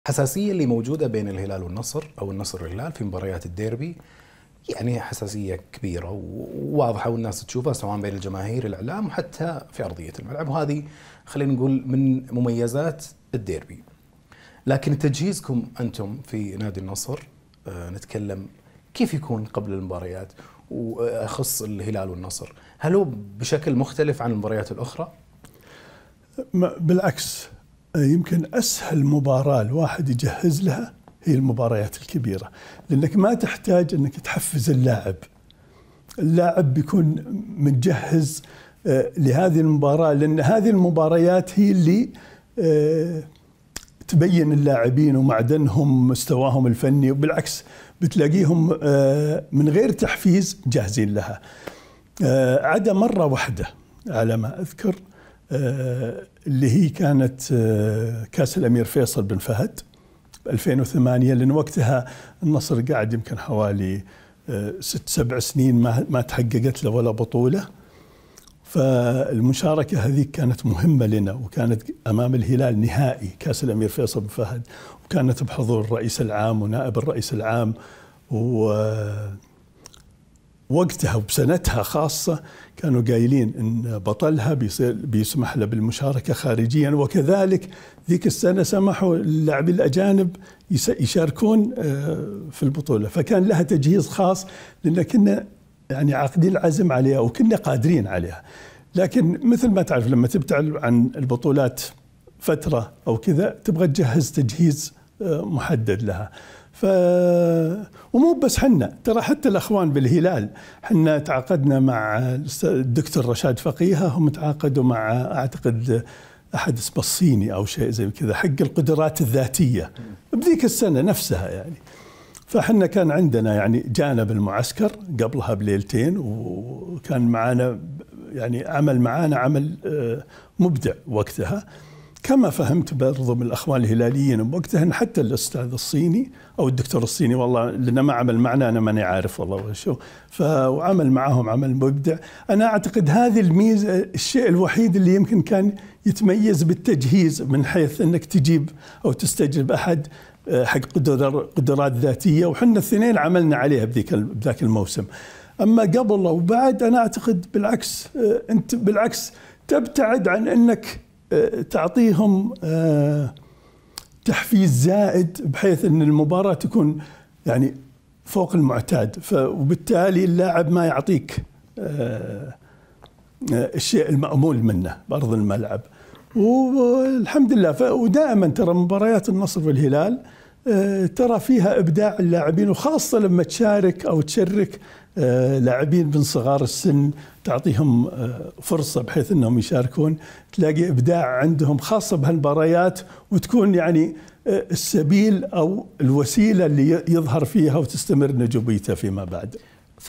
الأمير وليد بن بدر بن سعود: مباريات النصر والهلال دائما فيها إبداع للاعبين
مباريات النصر والهلال دائما فيها إبداع..

"أسهل المباريات في تجهيز اللاعبين هي المباريات الكبيرة، عدا مرة واحدة أمام الهلال في 2008"

الأمير وليد بن بدر بن سعود